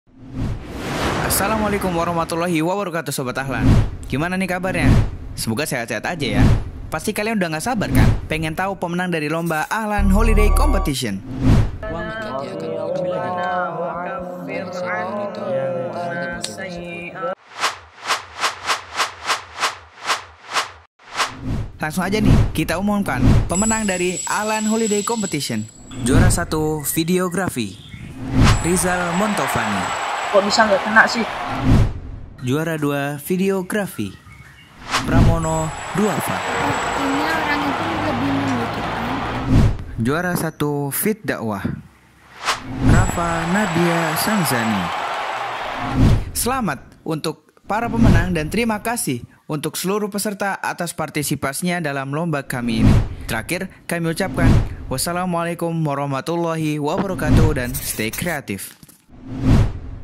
PENGUMUMAN PEMENANG AHLAN TV HOLIDAY COMPETION
Setelah melakukan banyak pertimbangan dan pemilihan, akhirnya yang ditunggupun terbit!

32 Peserta hanya 3 dari mereka yang terpilih!!!

Siapakah Mereka???